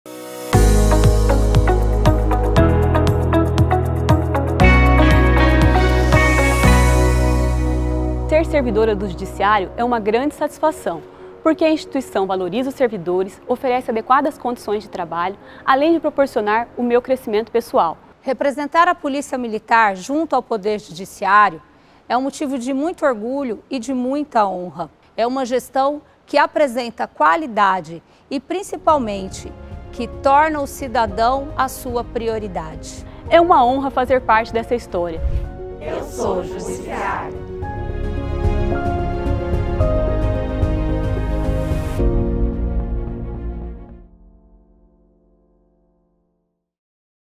150 Anos - Eu Sou o Judiciário | Jane de Sousa Melo e Bruna Thaisa Dias
📜 15 décadas de trajetória e transformação

🪶 150 anos que exigiram passos firmes e muito trabalho daqueles que começaram e dos que hoje continuam a escrever diariamente essa história.

⭐ Hoje você conhecerá os rostos por trás da Coordenadoria Militar e Coordenadoria Administrativa no ano em que o Tribunal de Justiça de Mato Grosso celebra o sesquicentenário!

🔸 Coordenadora Militar | Coronel Jane de Sousa Melo

🔸 Coordenadora Administrativa | Bruna Thaisa Dias Penachioni Ivoglo